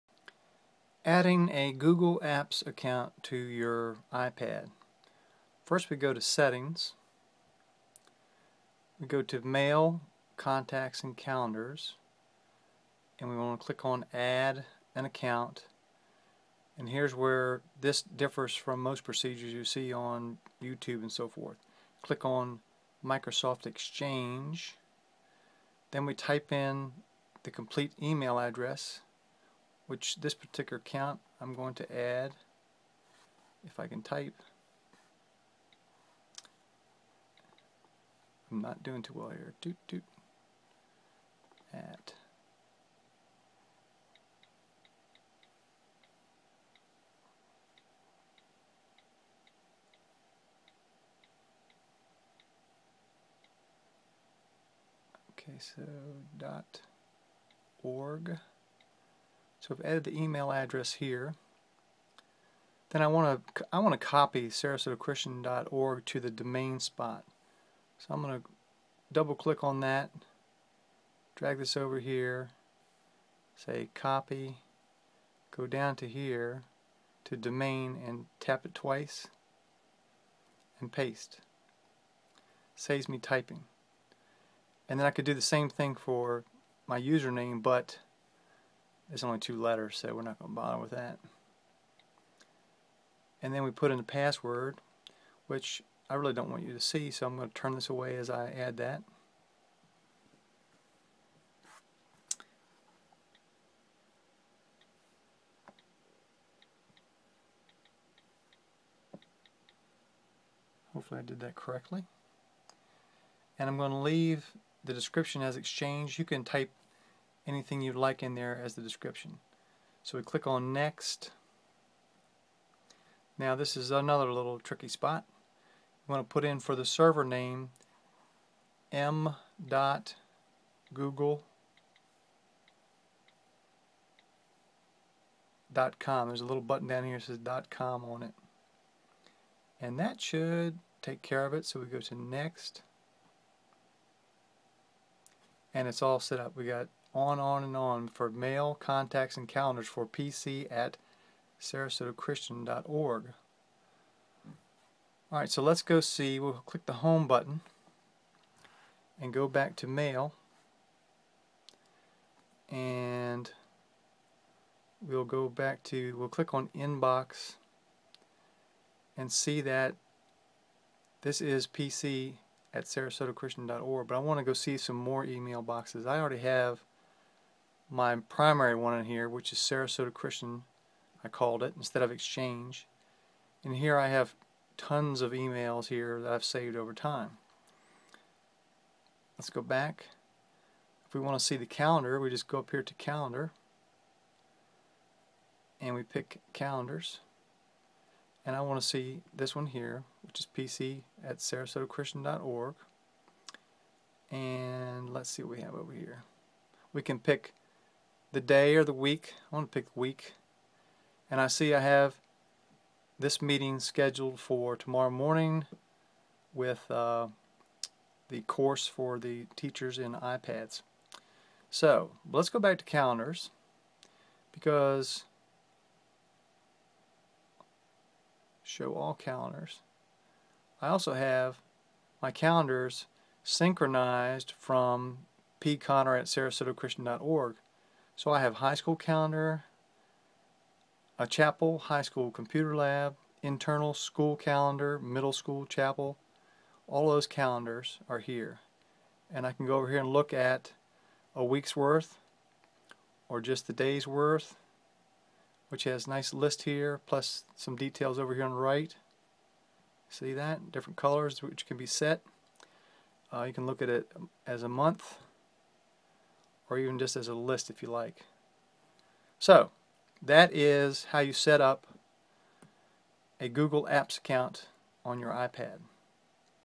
How to add a Google Apps account to your iPad
Must be completed before syncing multiple calendars.  Server is m.google.com.